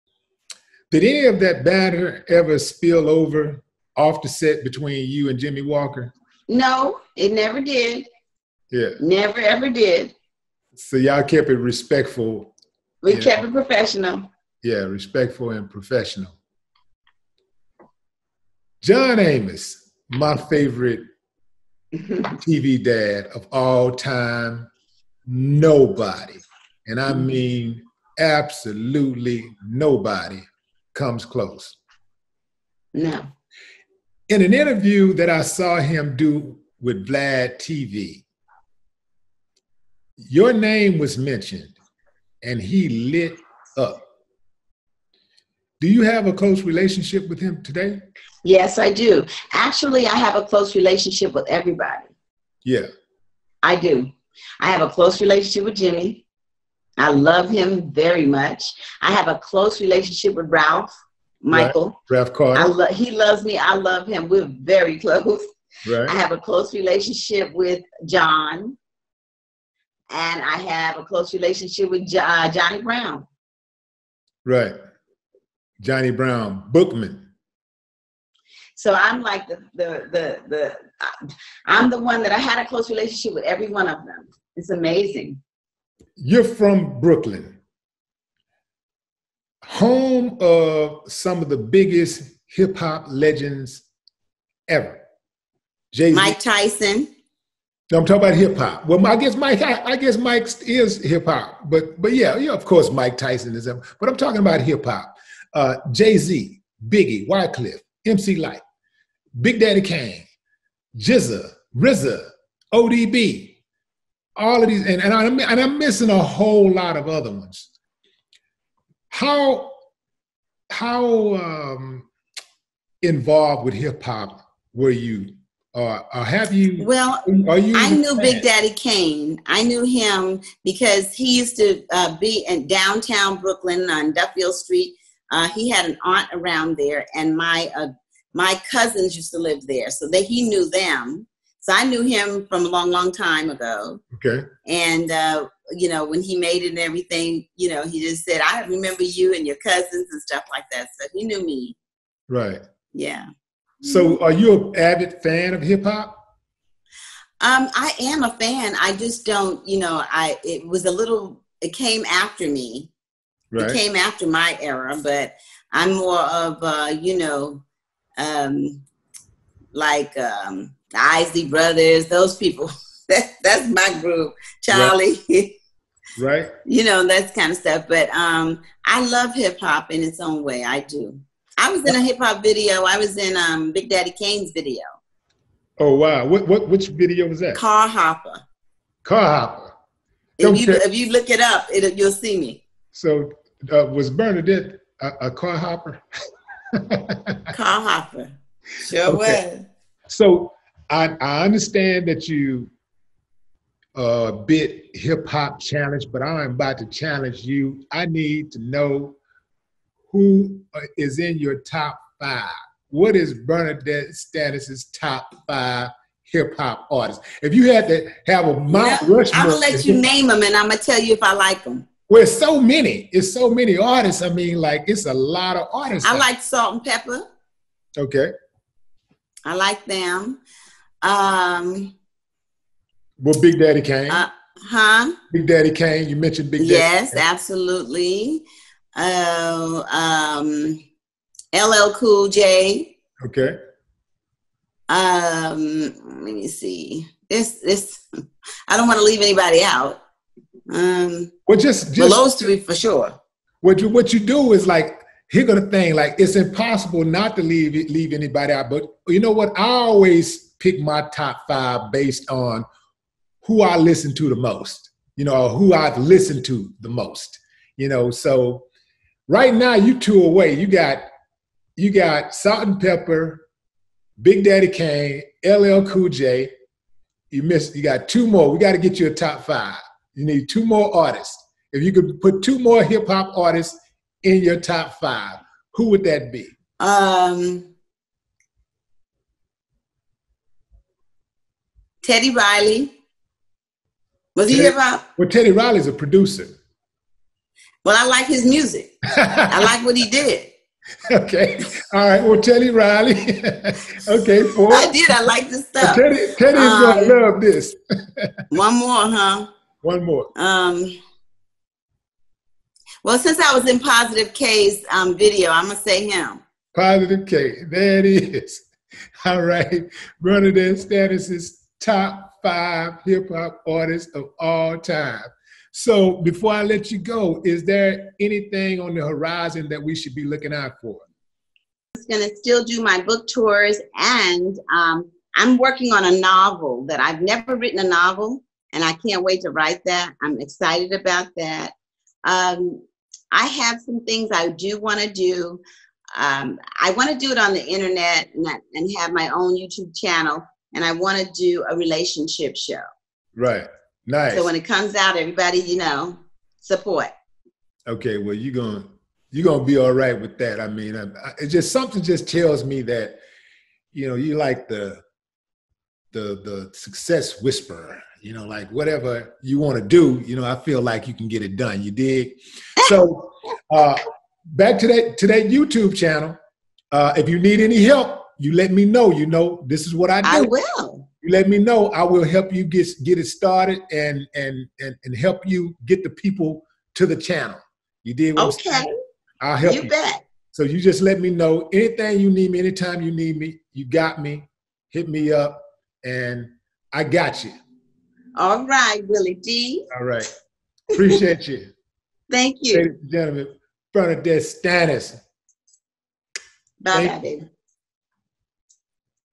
Top 5: You'll Never Guess Who "Good Times" Actress BernNadette Stanis aka Thelma Picked (Part 8)
Attention Subs!!!

$WillieDLive

$WillieDLive